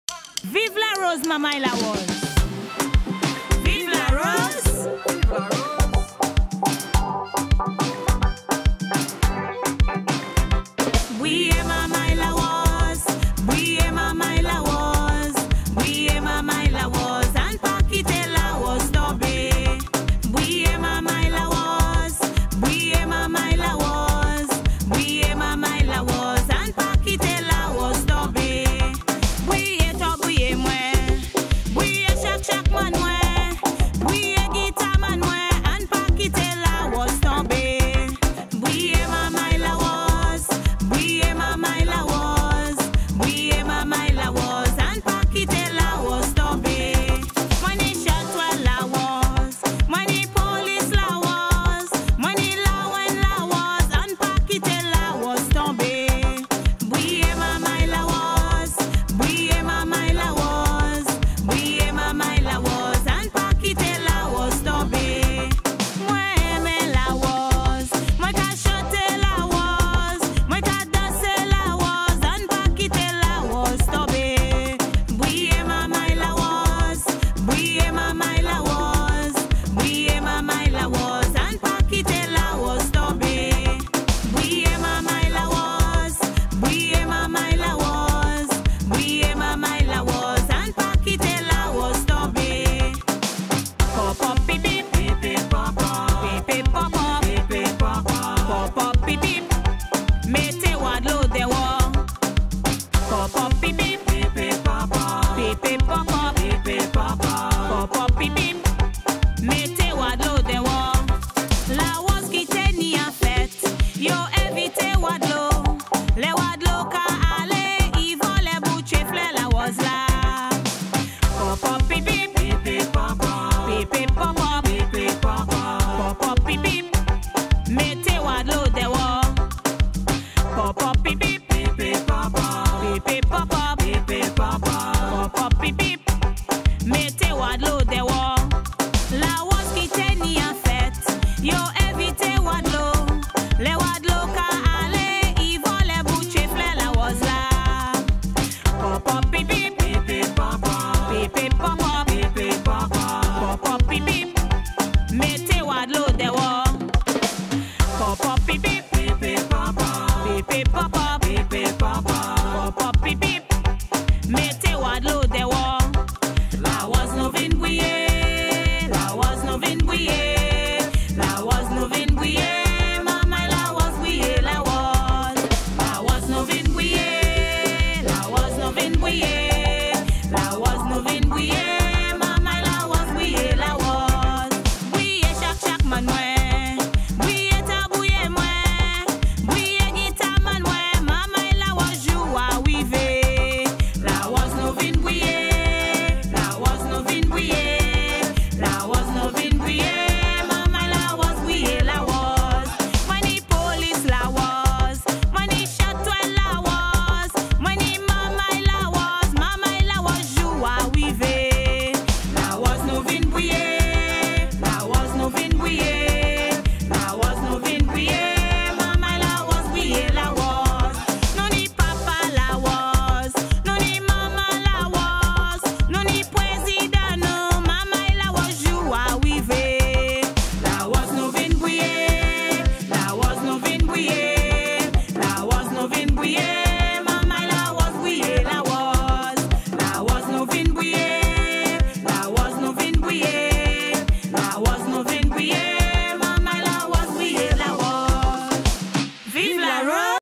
La Rose Medley Madame Belle 2018
A medley of 3 La Rose songs sang by Madame Belle of the Monchy La Rose group in Saint Lucia.